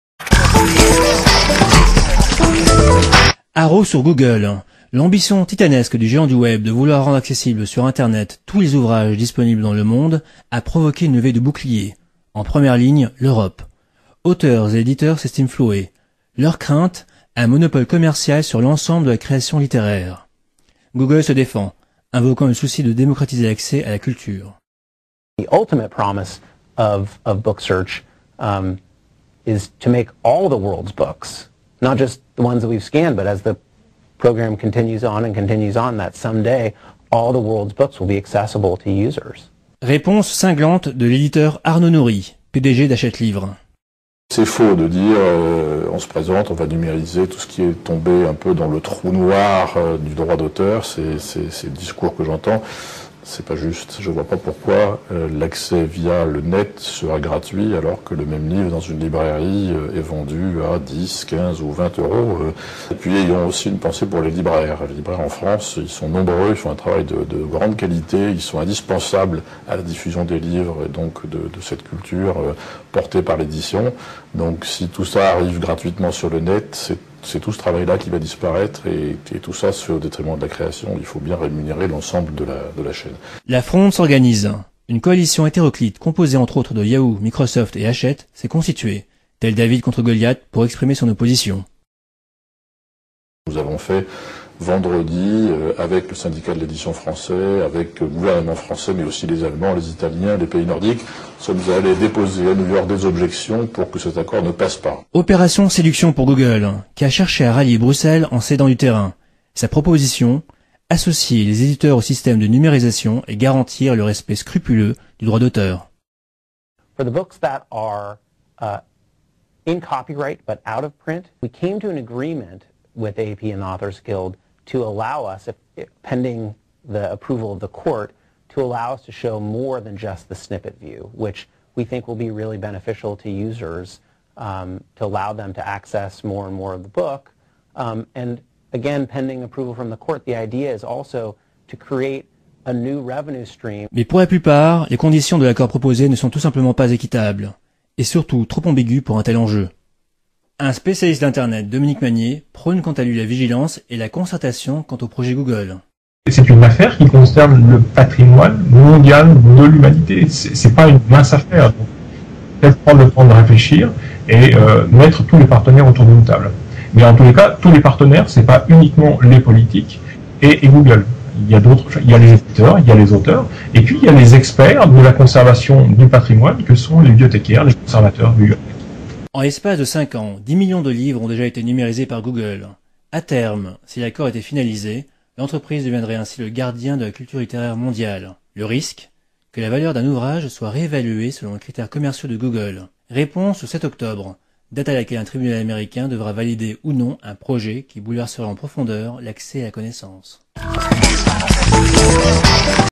[40] Google à l'assaut des livres
Septembre 2009 : reportage réalisé sur l'actualité d'Internet et consacré au projet de Google Books de vouloir numériser l'ensemble des livres disponibles dans le monde (version brute du sujet, avant doublage du sonore anglais, nouveau mixage et diffusion sur France24).